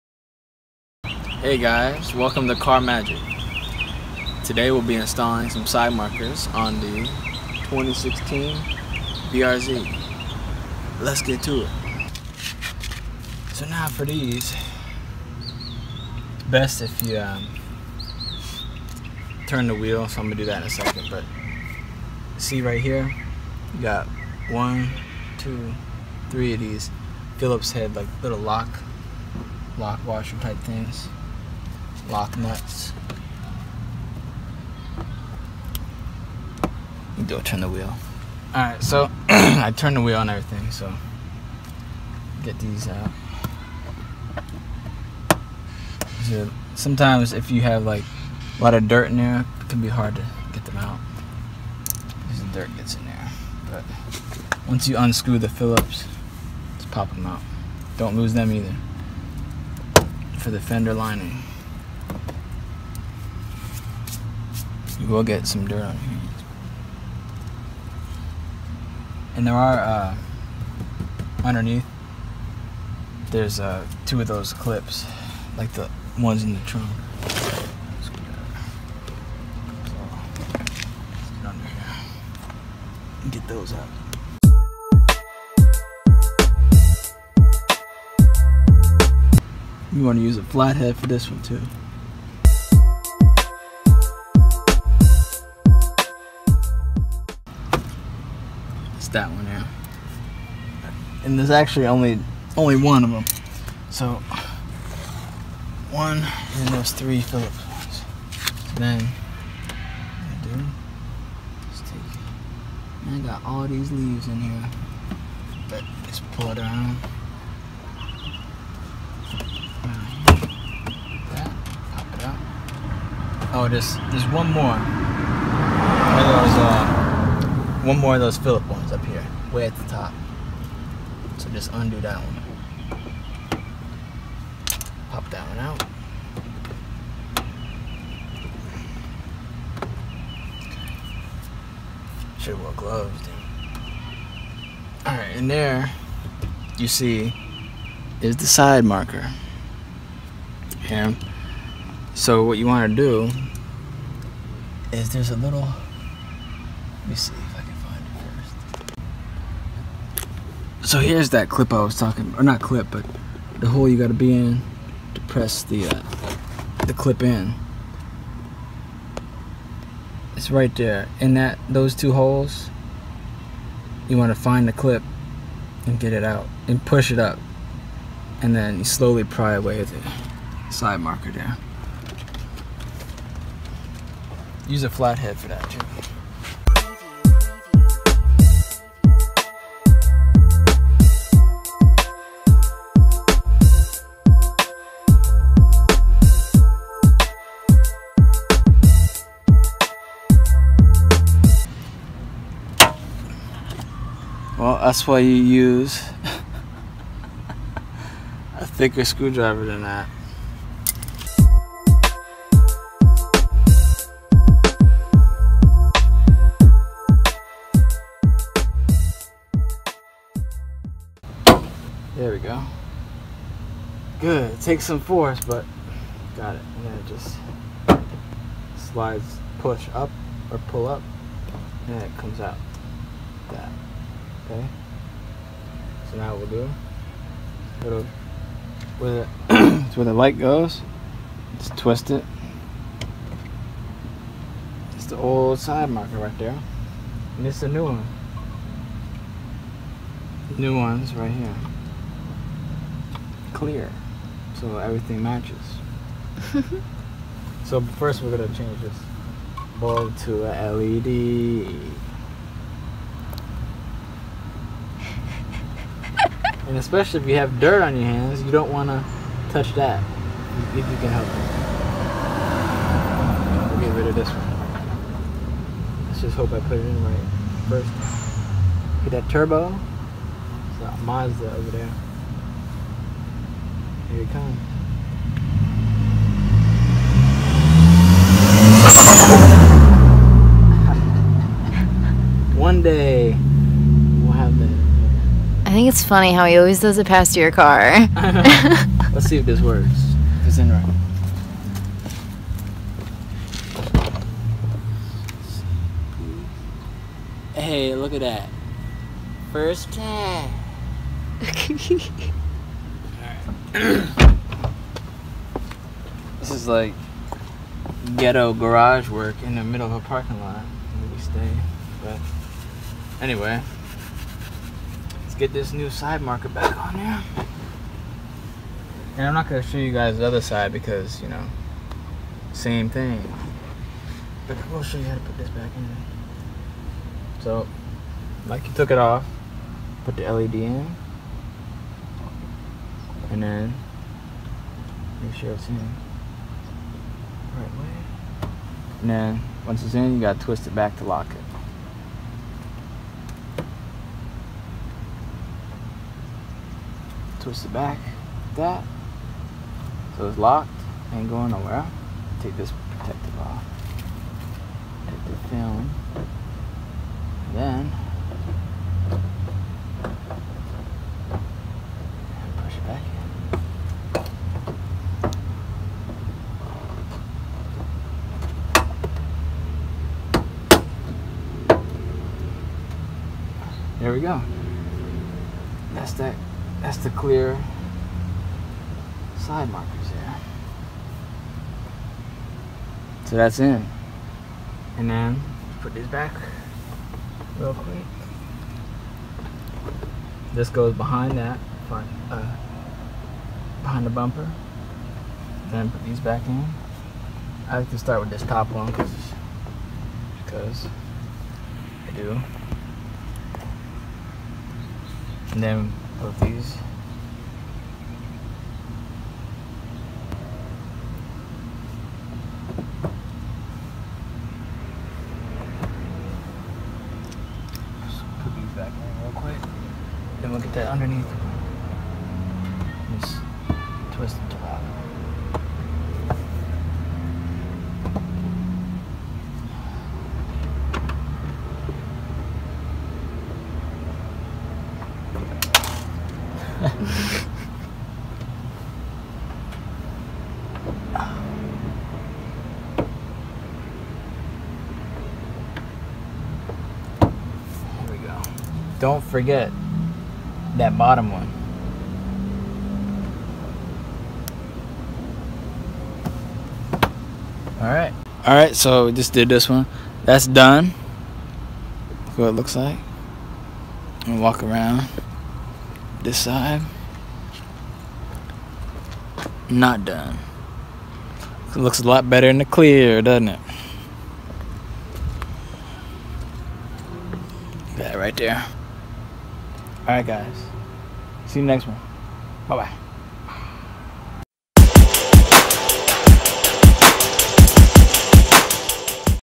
[How To]: Change Side Markers on 2016 Subaru BRZ
This video explains how to remove and replace your side markers and bulbs on a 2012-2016 BRZ.

Side Markers Used

Bulbs Used